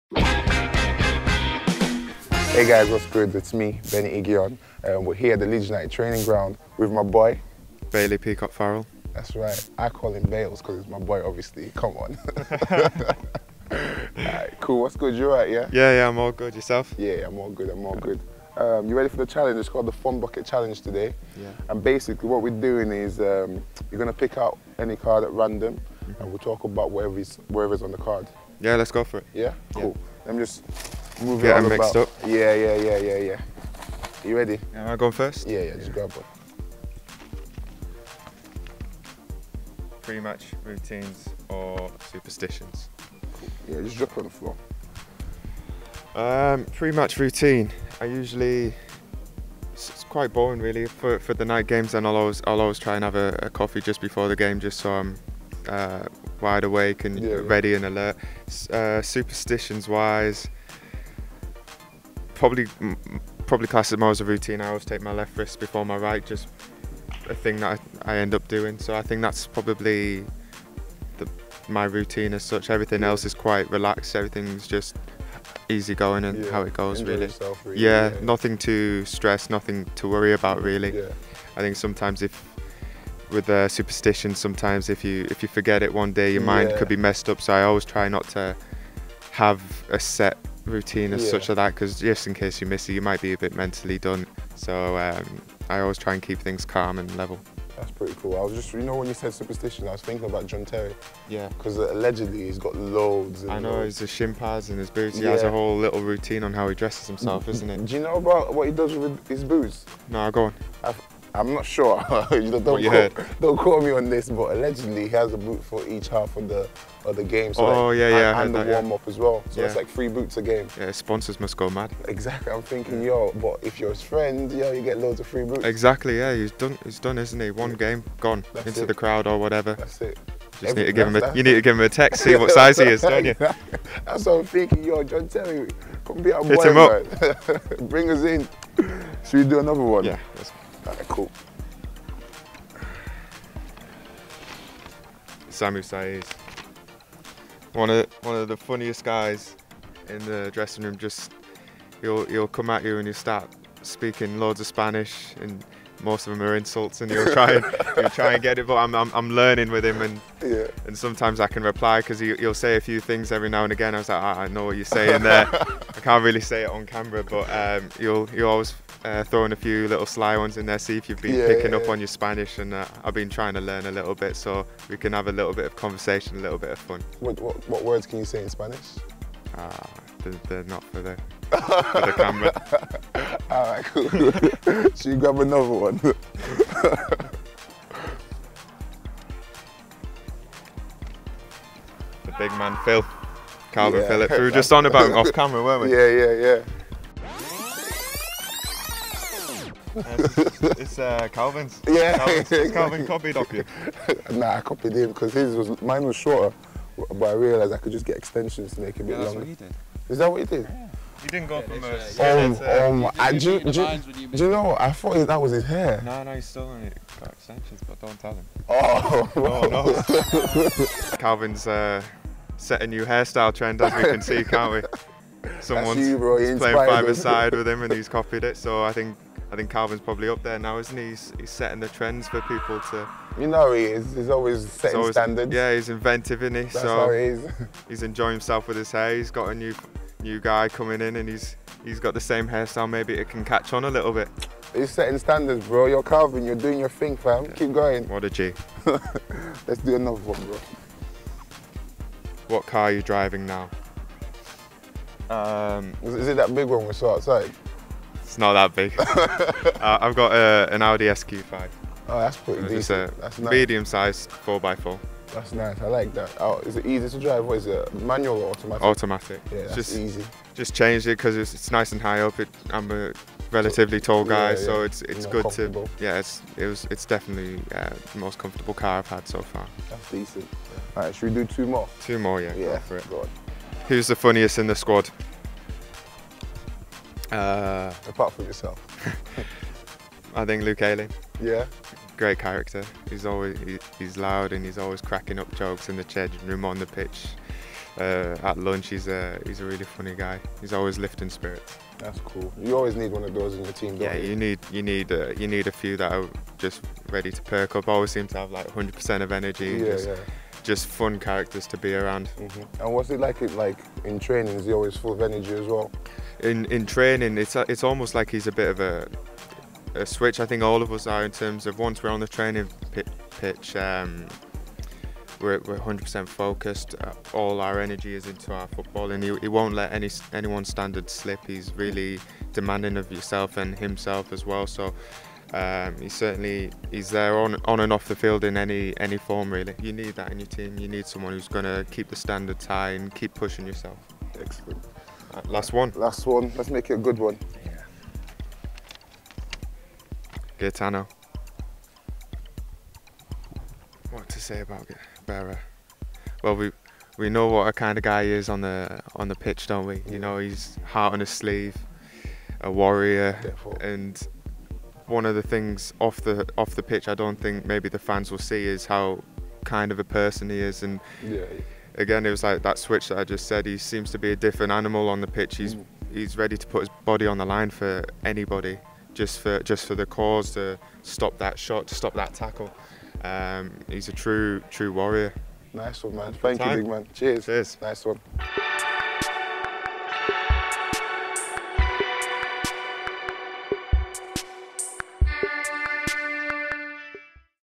FALSE HAIR AND SPANISH INSULTS 🤔 BAILS TAKES ON THE FUN BUCKET CHALLENGE⚽️
Watch The Peacock spill the beans on all of his team mates "they're extensions, they ain't real"